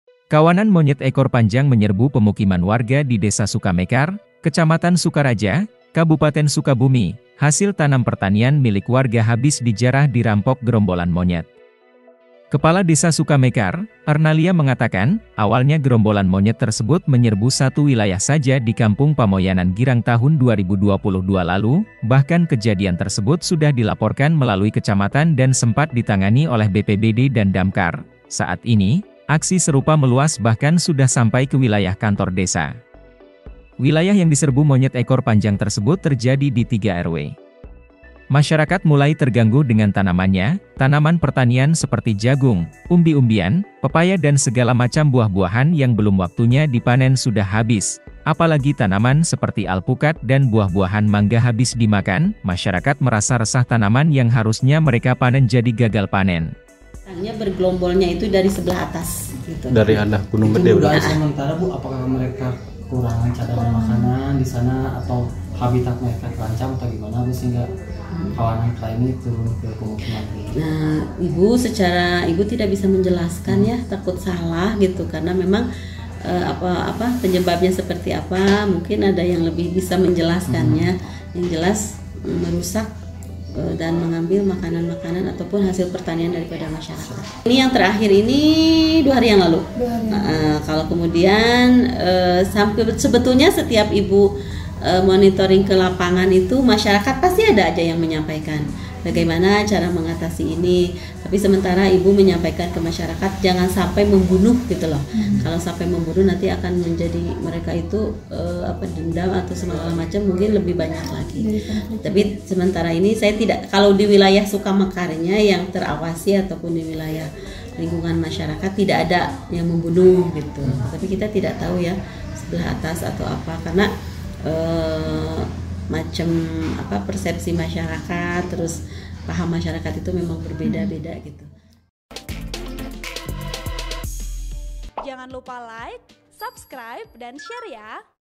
SATU KAMPUNG DI SUKABUMI 'DIRAMPOK' & DISERANG GEROMBOLAN MONYET, HASIL BUMI HABIS DIJARAH
TRIBUNJABARVIDEO- Kawanan monyet ekor panjang menyerbu pemukiman warga di Desa Sukamekar, Kecamatan Sukaraja, Kabupaten Sukabumi.

Hasil tanam pertanian milik warga habis dijarah dirampok gerombolan monyet.

Kepala Desa Sukamekar, Ernalia mengatakan, awalnya gerombolan monyet tersebut menyerbu satu wilayah saja di Kampung Pamoyanan Girang tahun 2022 lalu

Bahkan kejadian tersebut sudah dilaporkan melalui kecamatan dan sempat ditangani oleh BPBD dan Damkar.

Saat ini, aksi serupa meluas bahkan sudah sampai ke wilayah kantor desa.

Wilayah yang diserbu monyet ekor panjang tersebut terjadi di 3 RW. Masyarakat mulai tergangguh dengan tanamannya.

Tanaman pertanian seperti jagung, umbi-umbian, pepaya dan segala macam buah-buahan yang belum waktunya dipanen sudah habis, apalagi tanaman seperti alpukat dan buah-buahan mangga habis dimakan.

Masyarakat merasa resah tanaman yang harusnya mereka panen jadi gagal panen.(*)